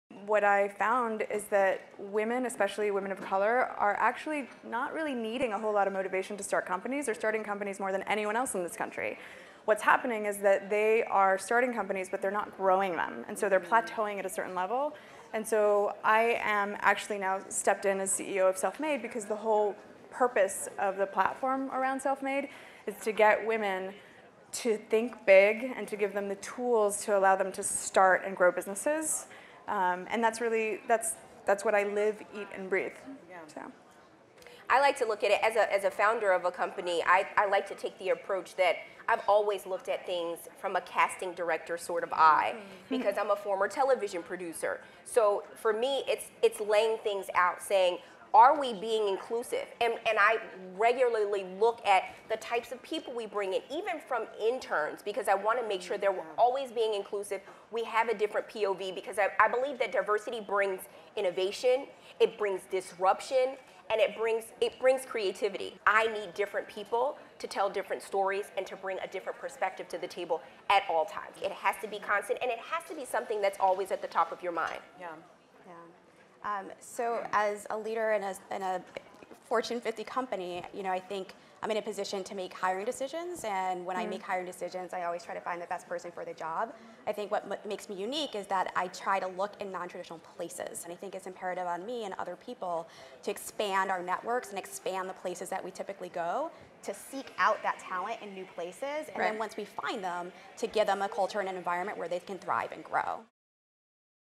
Tara Galvin, Nathalie Molina Niño, Ebony Lee and Rakia Reynolds on Growing Businesses
How can women start and grow companies?